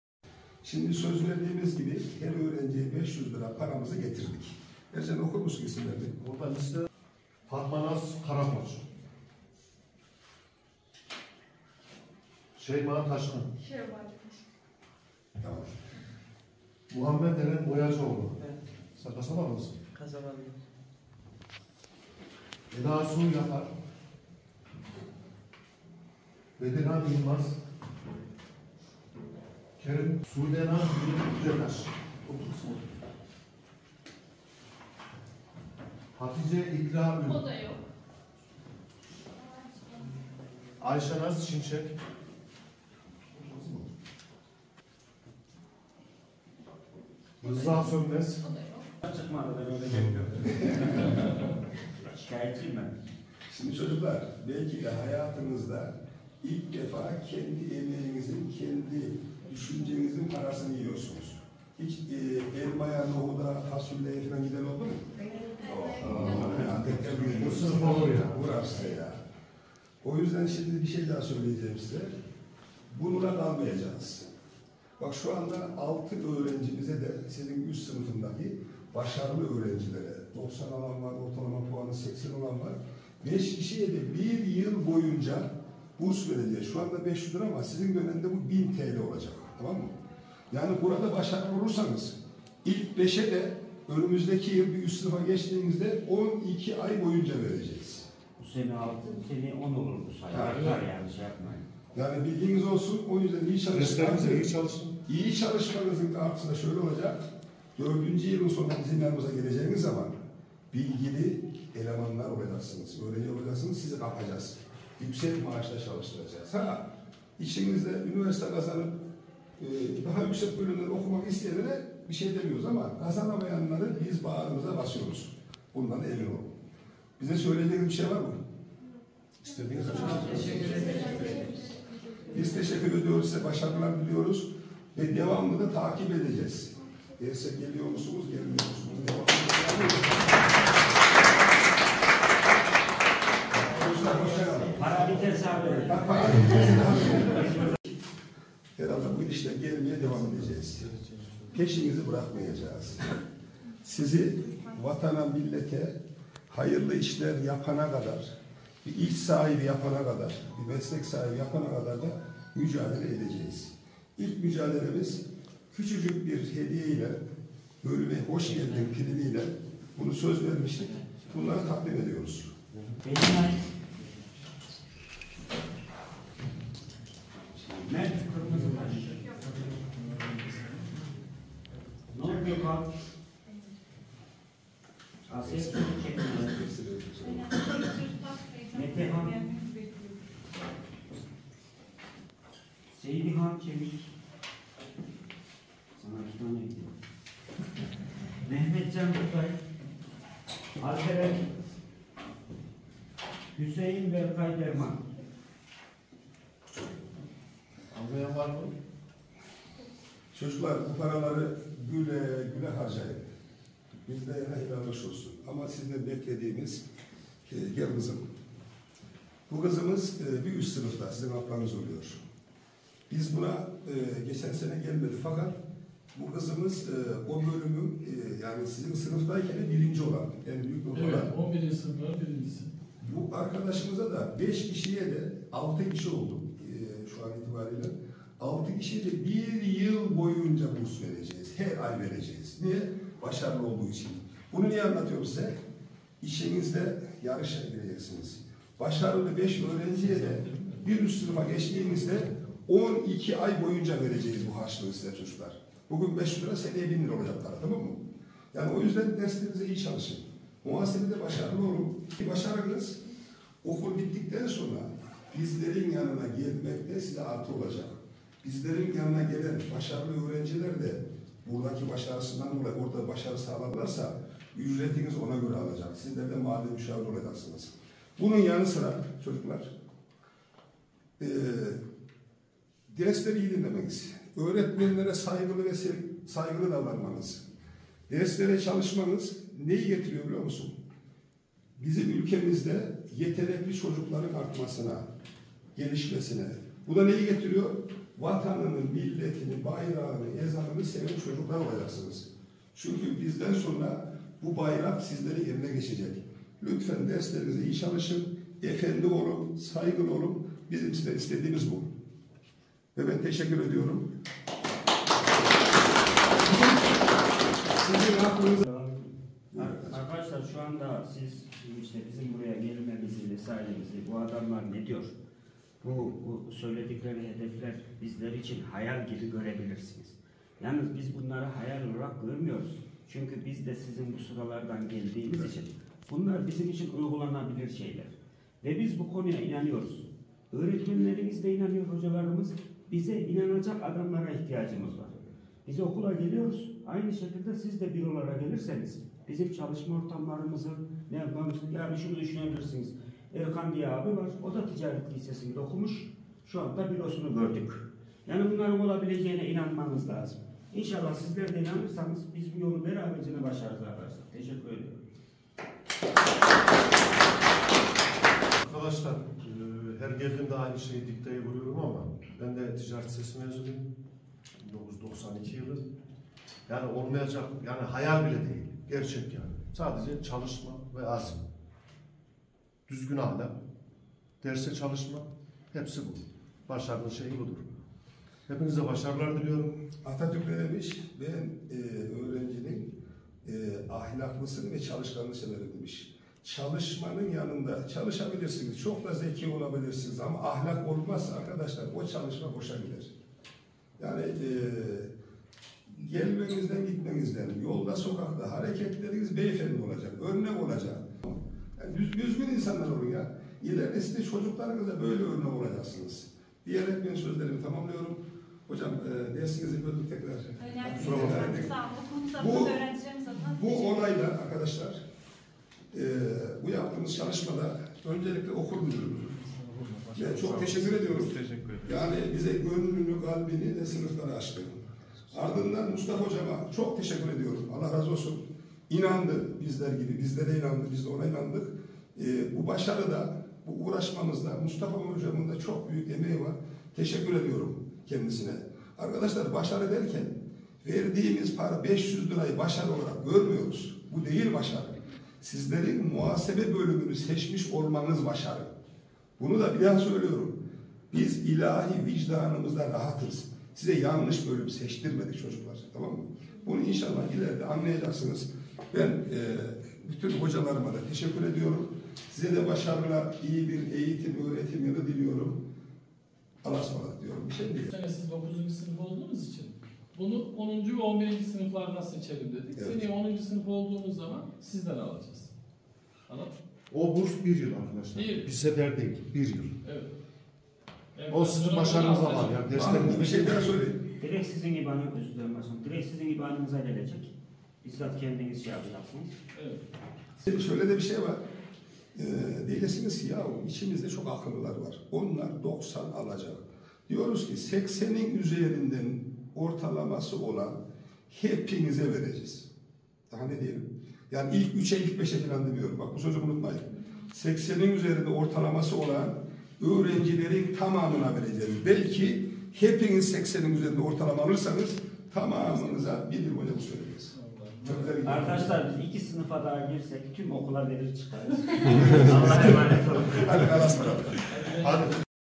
Başkan Ankaralı, sözünü tuttu: Karaman'da 42 öğrenciye burs verdi
SMMM Oda Başkanı Ali Ankaralı 21 Eylül 2023 tarihinde yaptığı basın açıklamasında Ticaret Lisesine kayıt yaptıran öğrencilerden muhasebe bölümünü seçen bütün öğrencilere bir defaya mahsus "bölüme hoş geldin" pirimi, ayrıca bir üst sınıfın en başarılı 6 öğrencisine de bir yıl boyunca burs vereceklerini söylemişti.